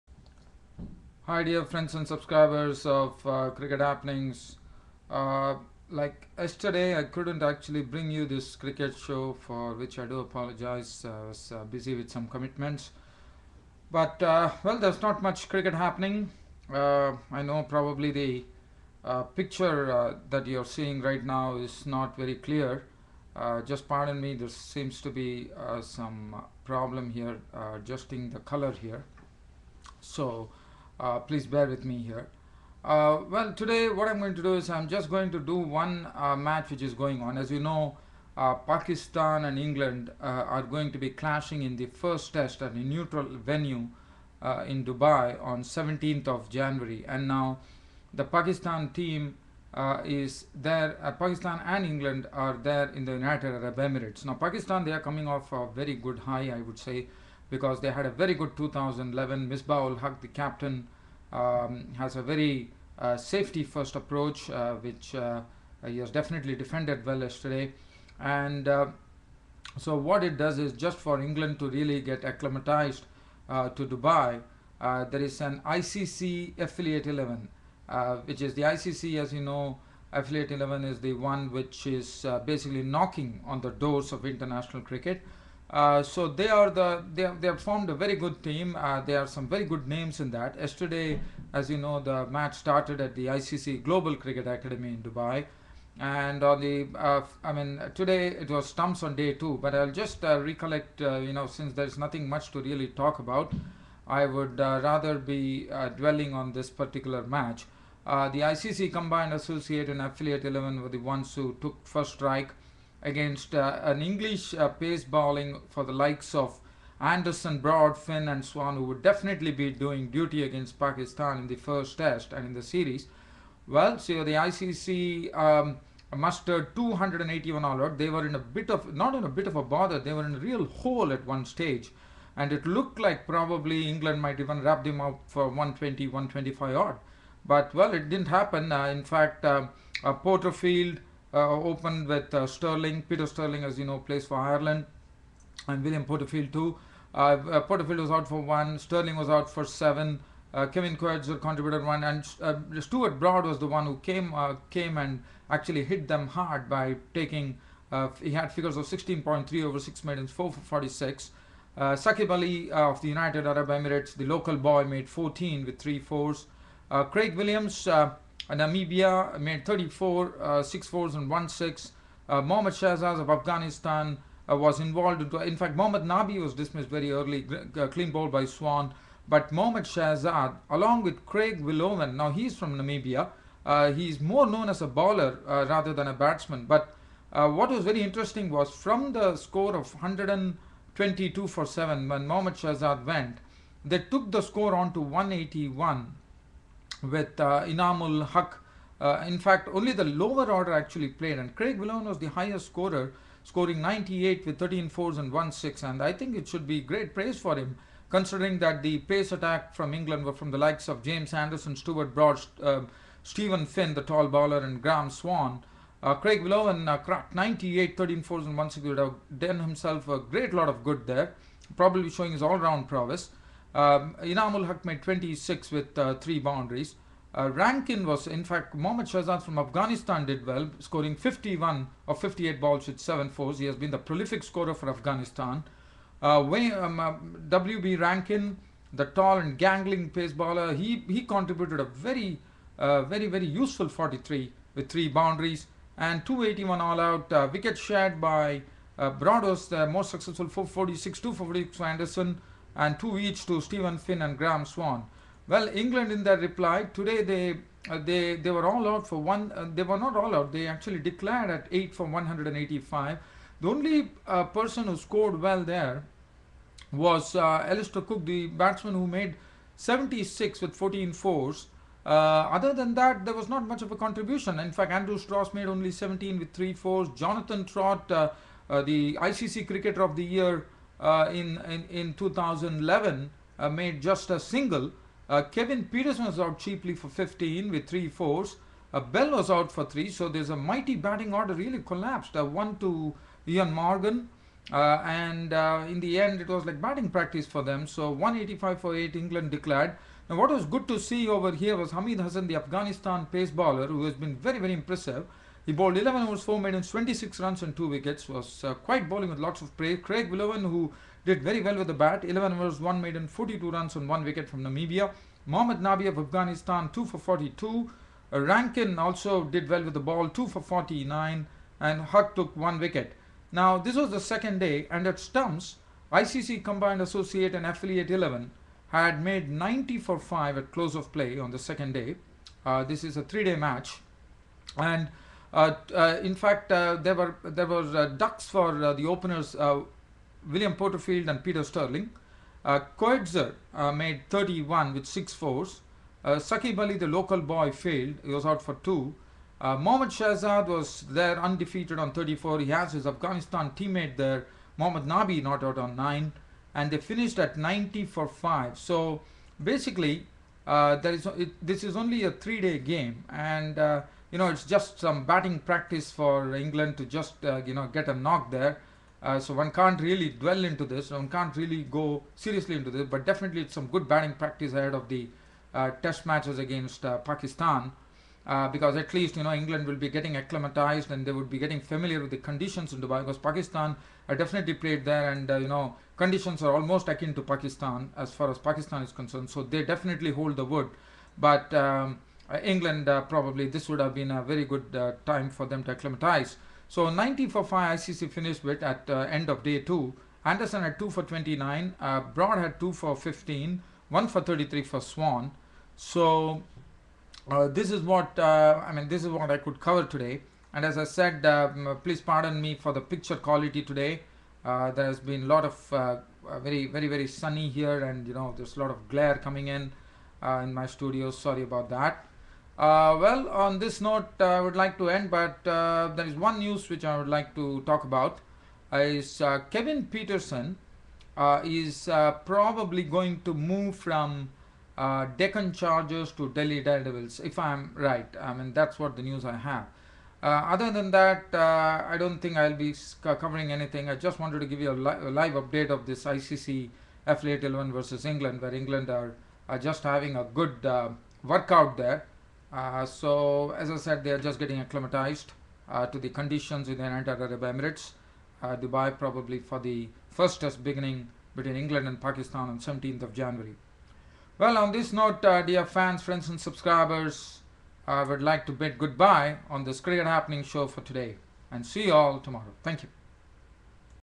cricket happenings England versus ICC & affilate nations team in UAE
ICC XI 90 FOR 5 AT STUMPS ON DAY 2 AGAINST ENGLAND IN DUBAI AFTER ENGLAND DECLARED AT 185 FOR 8 EARLIER. ICC BOWLERS DOING A GOOD JOB WITH THE BALL. ALISTAIR COOK SCORED 76